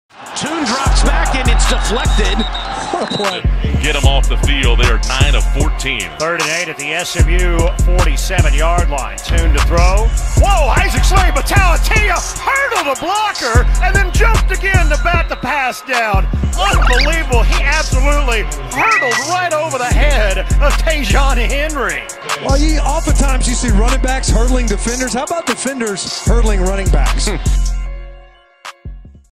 Isaac Slade-Matautia Insane Jump Over Running Back - SMU at Houston 2021
Follow the journey on social media @SMUFB